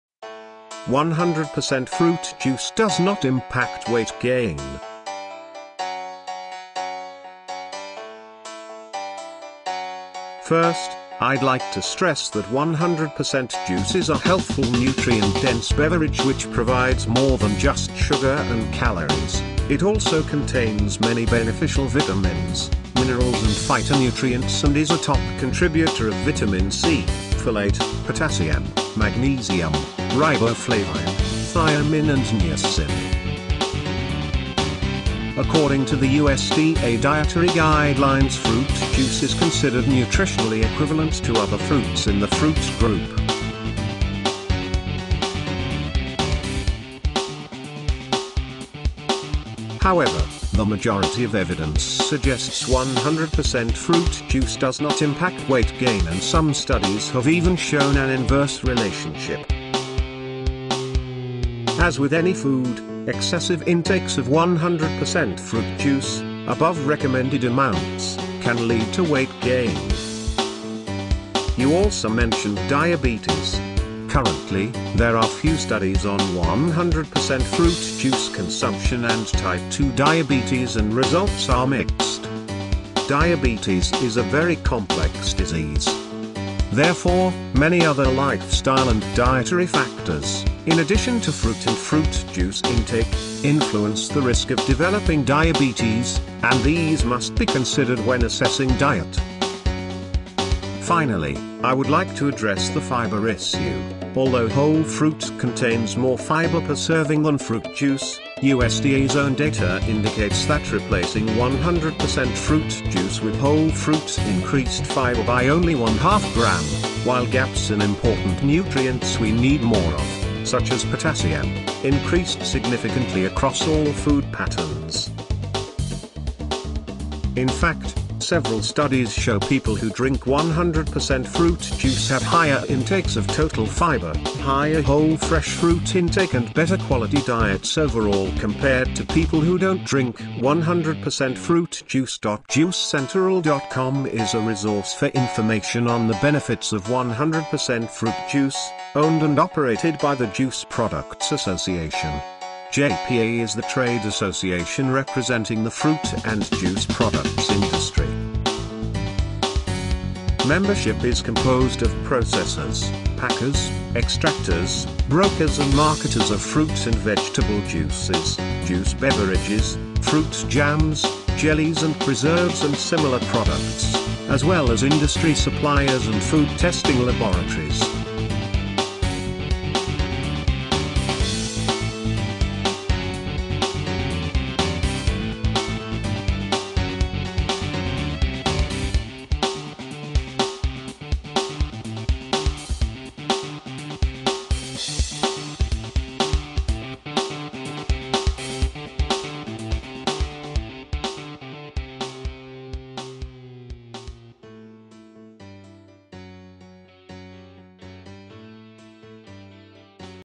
100 Percent Fruit Juice Does Not Impact Weight Gain
0:07.8 100% Fruit juice does not impact weight gain
0:13.3 Fruit juice is a healthful nutrient dense beverage
which provides more than just sugar and calories.

0:29.9 According to the USDA Dietary
Guidelines fruit juice is considered nutritionally

0:51.8 suggests 100 percent fruit juice does not
impact weight gain and some studies have even shown an inverse relationship